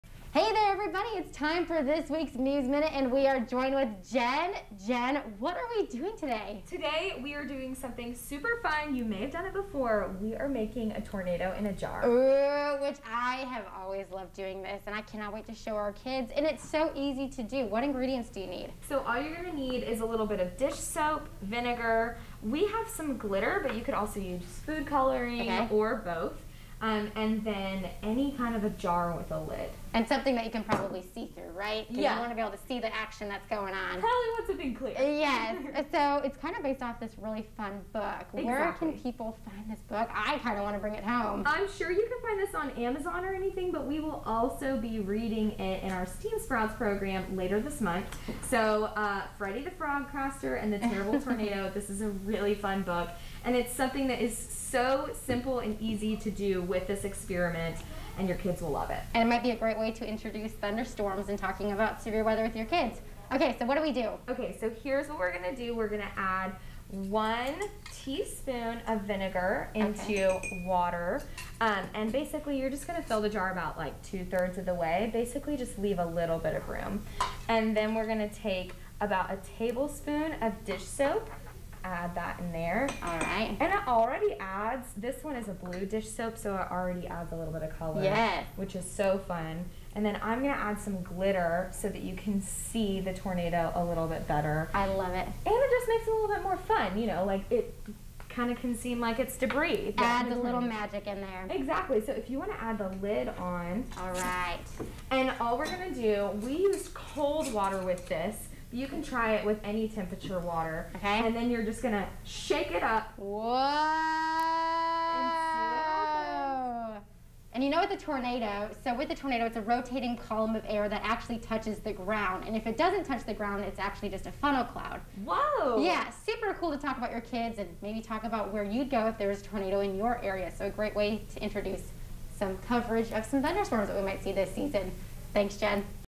10Learns: Tornado in a jar
In this week's Muse Minute, the Muse Knoxville shows 10news meteorologist Rebecca Sweet how to make a tornado in a jar.